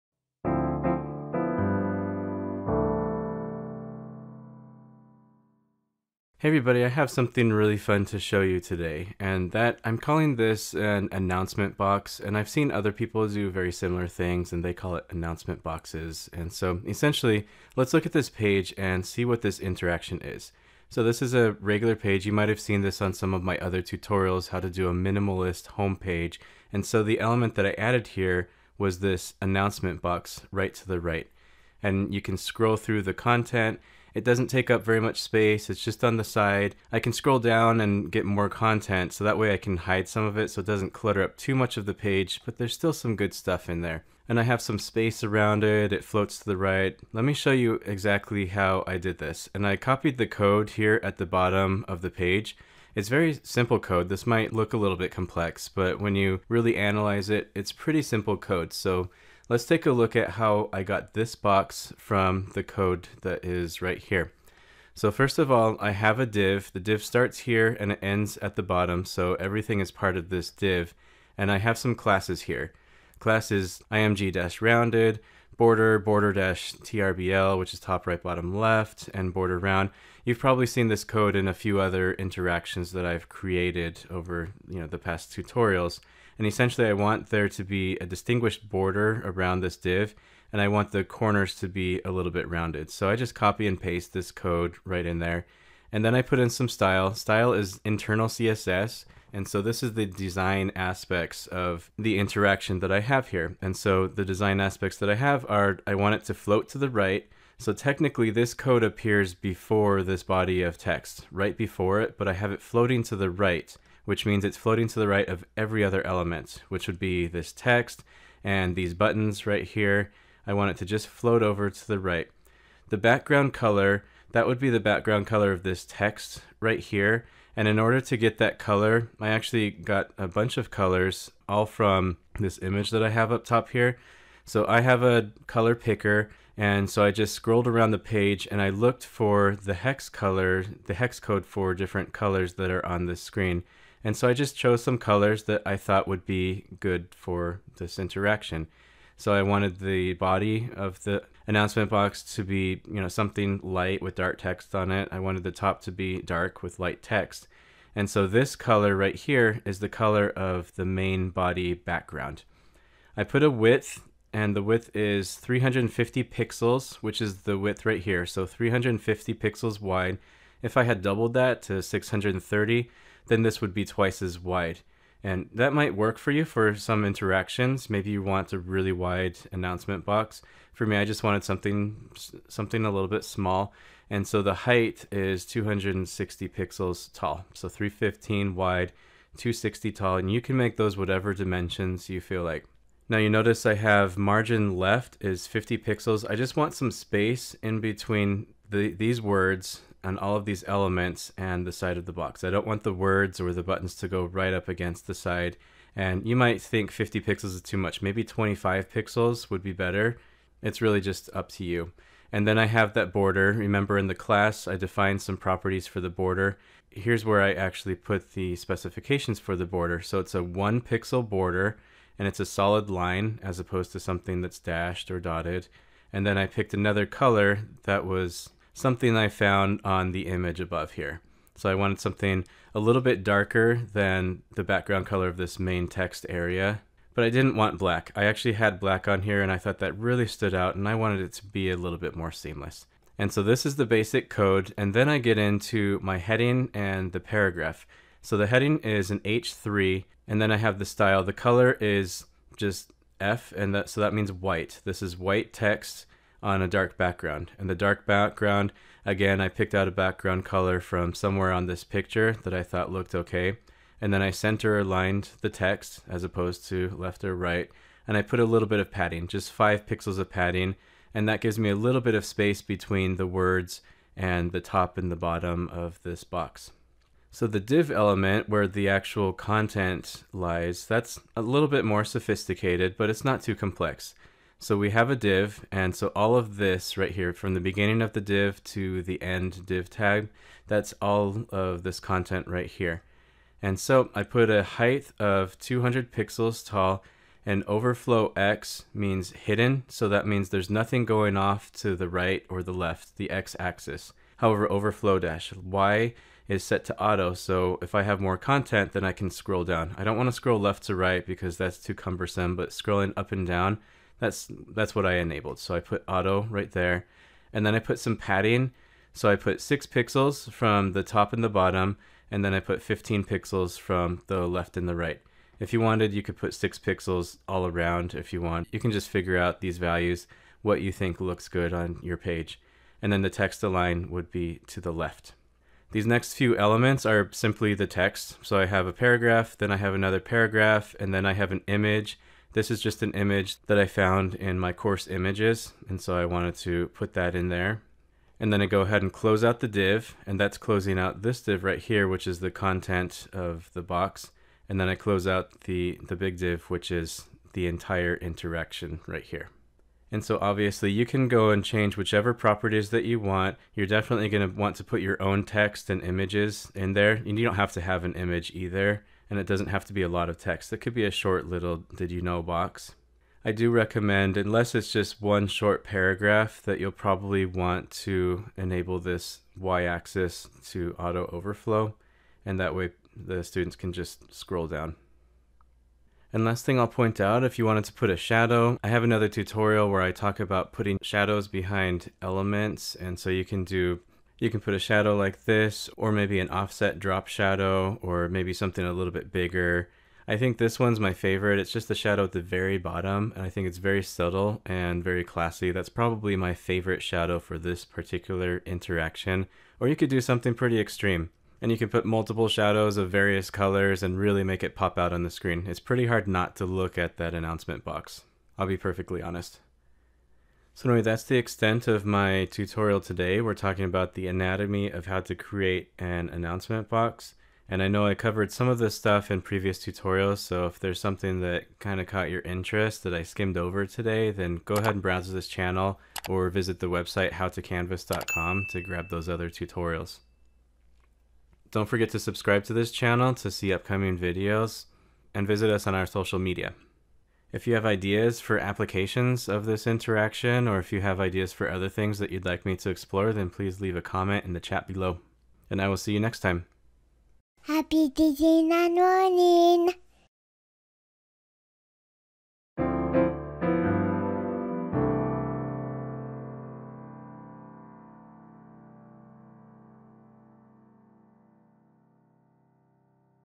Create an Announcement Box in Canvas
You can create a custom announcement or "Did you know??" box on any page in your Canvas course by using some simple HTML code.